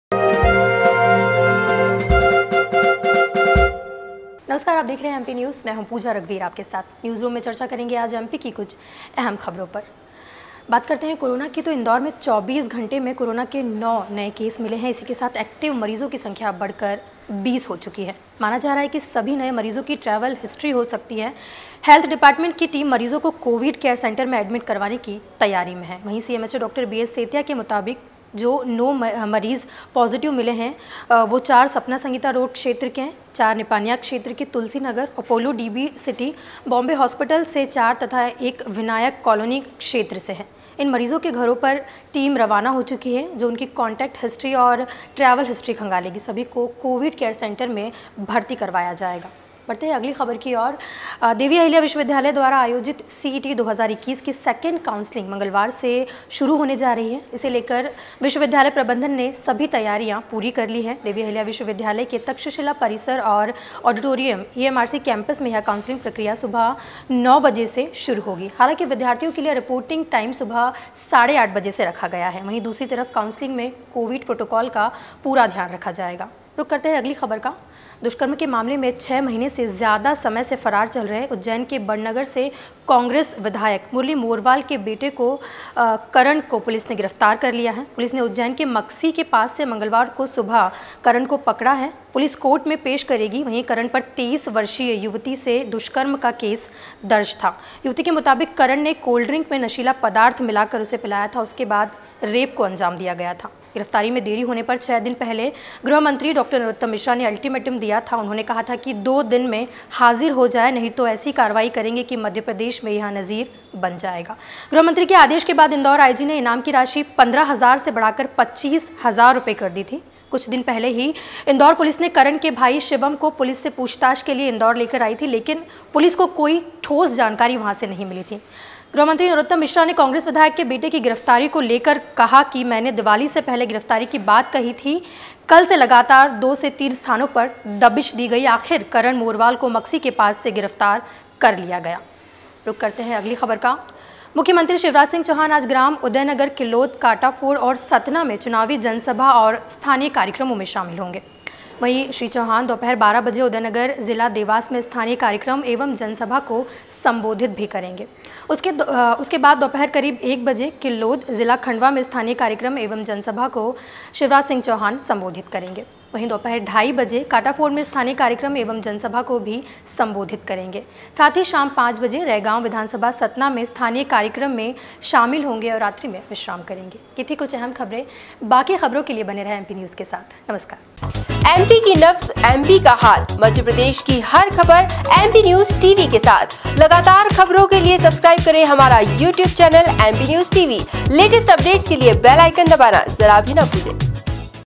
News Room: इंदौर में कोरोना के नए वैरिएंट AY-4 ने बढ़ाई टेंशन, इंदौर में सामने आए 9 नए मामले! MP News
News Room: इंदौर में कोरोना के नए वैरिएंट AY-4 ने बढ़ाई टेंशन, इंदौर में सामने आए 9 नए मामले! MP News TV